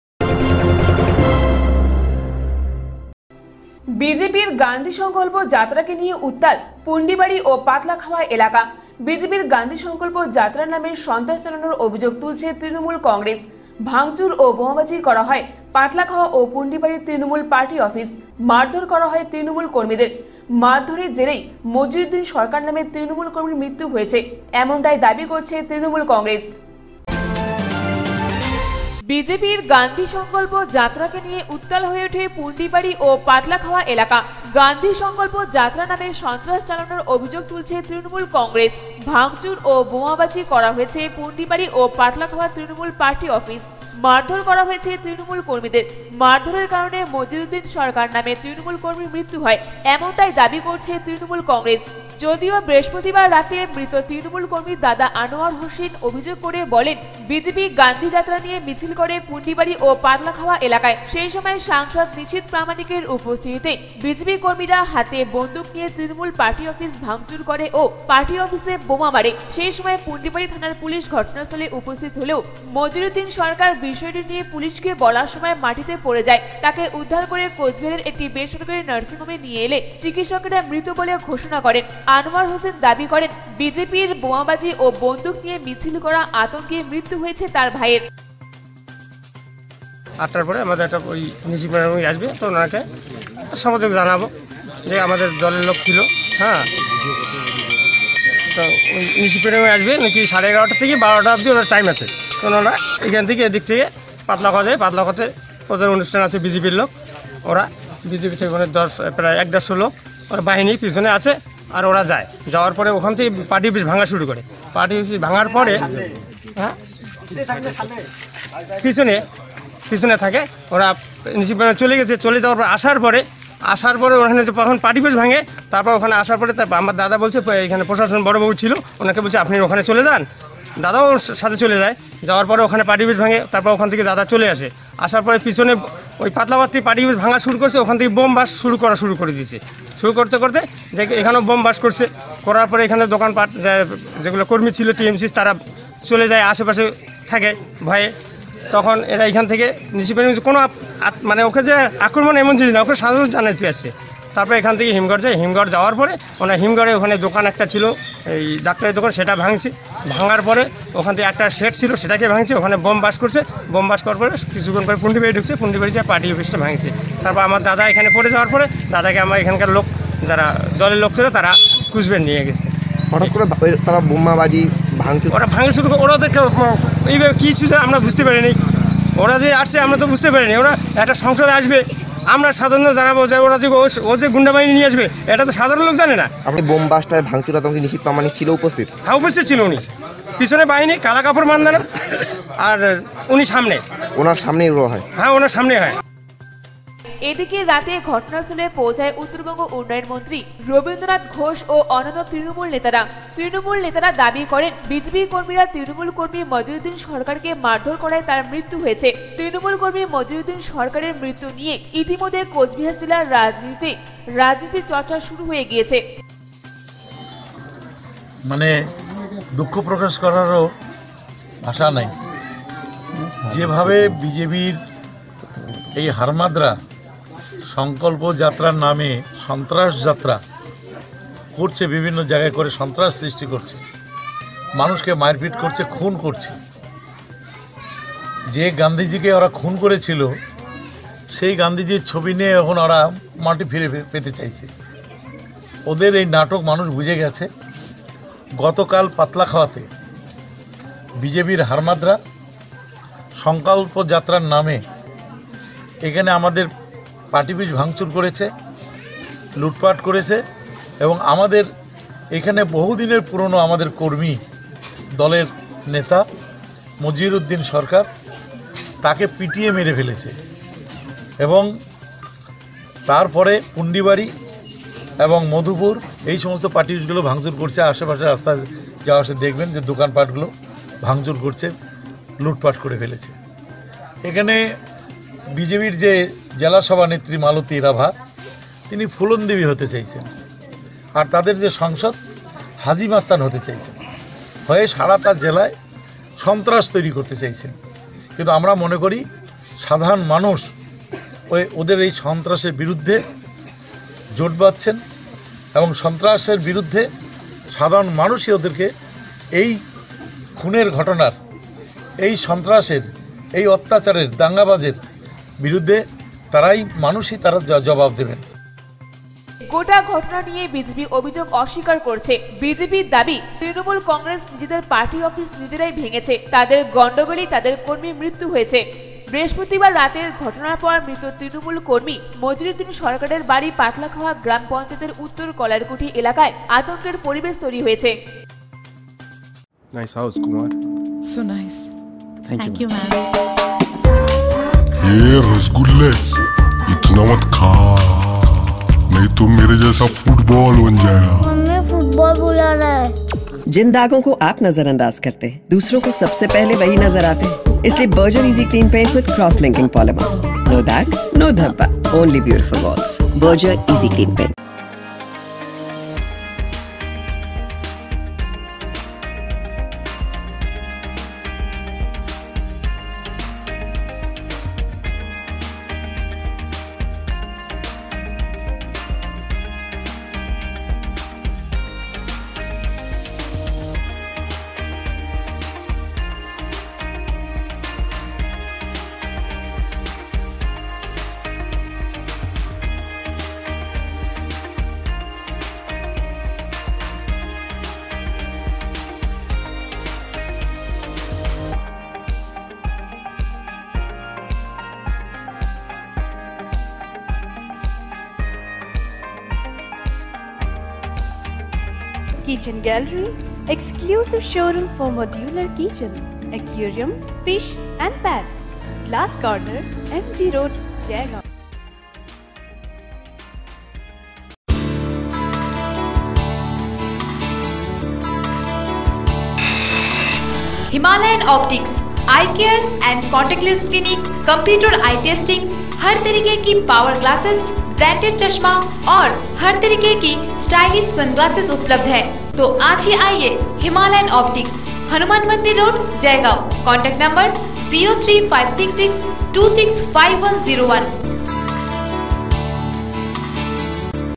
বিজেপি গান্ধী সংকল্প যাত্রাকে নিয়ে উত্তাল হয়ে ওঠে পুন্ডিবারী ও পাতলাখাওয়া এলাকা
বিজেপি গান্ধী সংকল্প যাত্রাকে নিয়ে উত্তাল হয়ে ওঠে পুন্ডিবারী ও পাতলাখাওয়া এলাকা, গান্ধী সংকলপ যাত্রা নামে সন্ত্রাস চালানোর অভিযোগ তুলছে তৃনমূল কংগ্রেস ,ভাংচুর ও বোমাবাজি  করা হয় পুন্ডিবারি ও পাতলাখাওয়া তৃণমূল পার্টি অফিস, মারধর করা হয় তৃণমূল কর্মীদের , মারধরের কারনে মজিরুদ্দিন সরকারনামে তৃণমূল কর্মী মৃত্যু হয়েছ, এমনটাই দাবি করে তৃণমূল কংগ্রেস